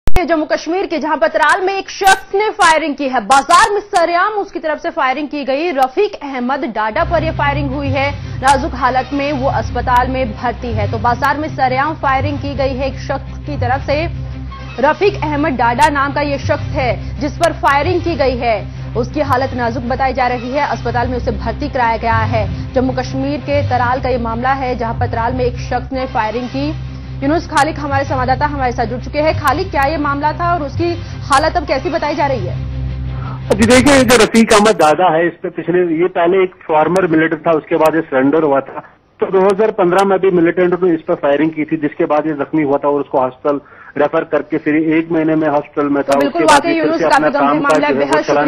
Former Militant Shot Dead In Tral Area of Pulwama: J&K
Rafiq Ahmad Bhat alias Dadaa, a former militant, was shot at and critically injured by the ultras at Tral Bala.